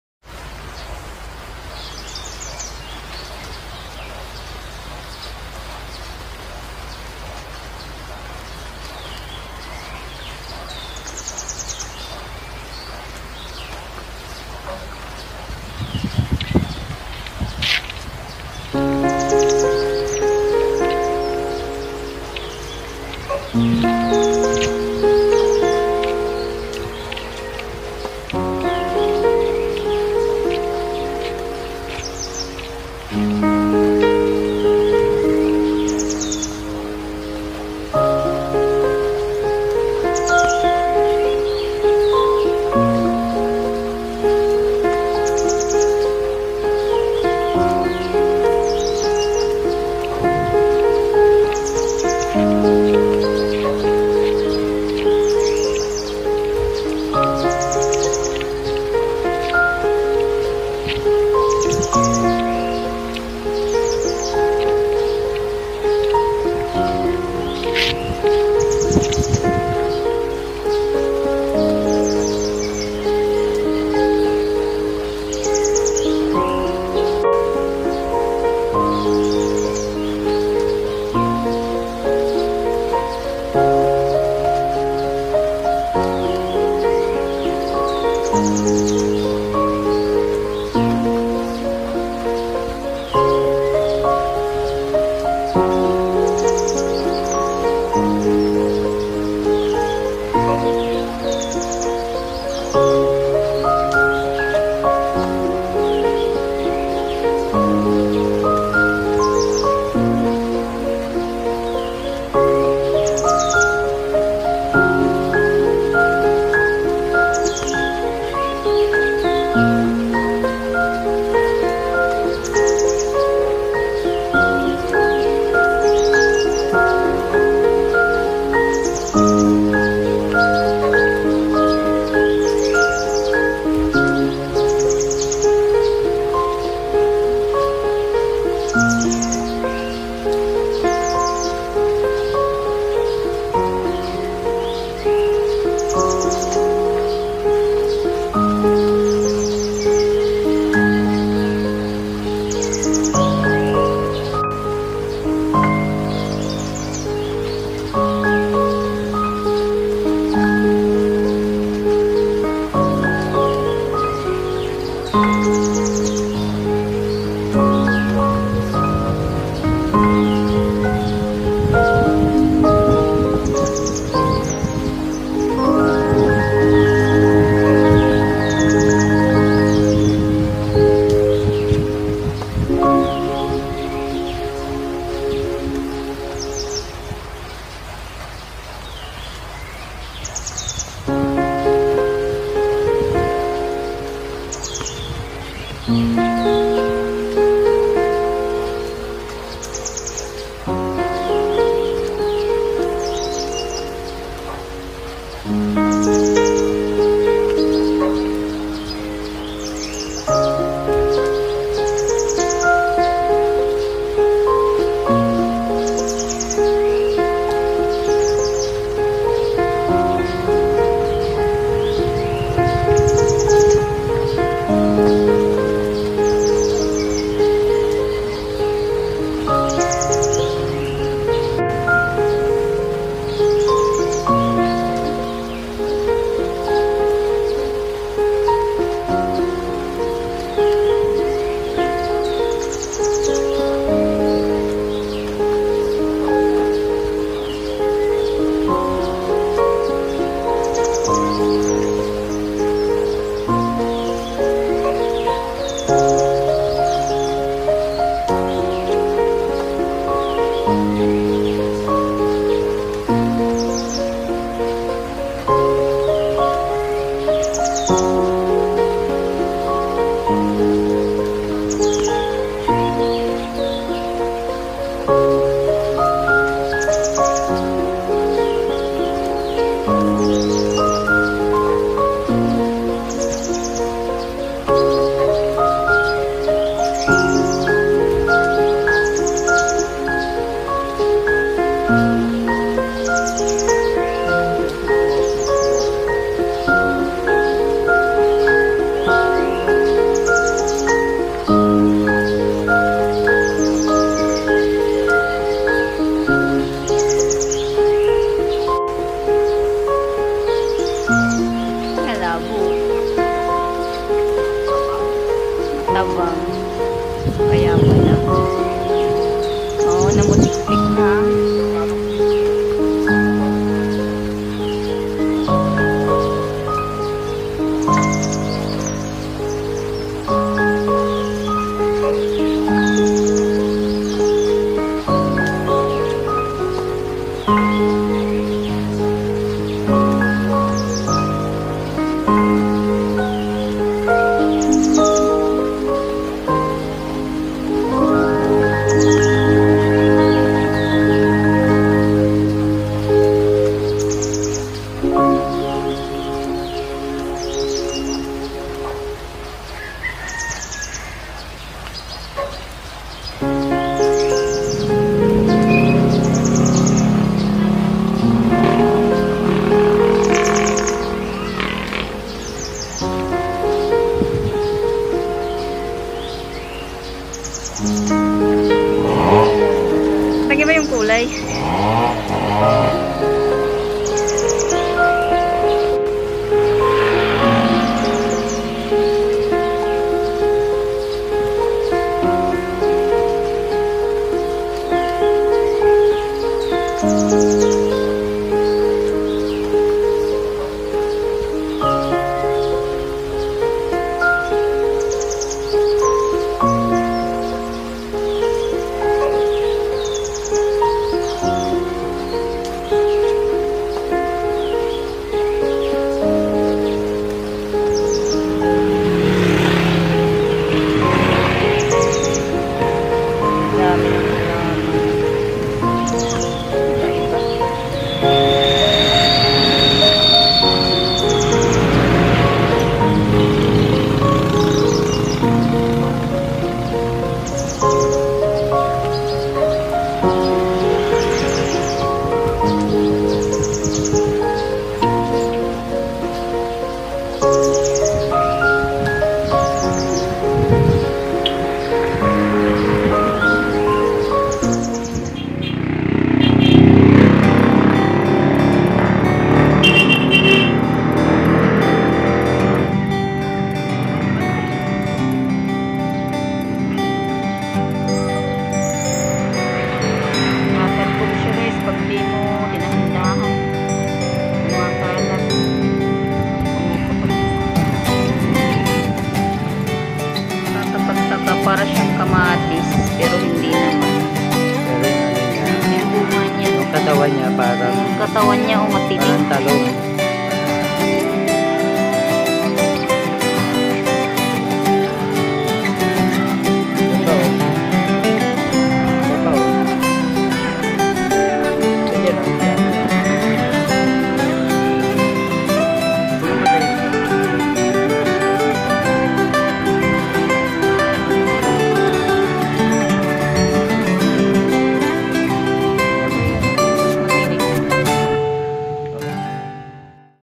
A Day in a life of Boris Gogol Channel: Walking around town of Siniloan Laguna. Flowers and Plants
This is a beautiful place in the town of Siniloan, Laguna Philippines. I will virtual tour you to the beautiful gardens of this place. A place with so many varieties of flowers and plants. Situated near mountain ranges and fresh water rivers, this town is full of its natural beauty. Come and join me on this virtual tour.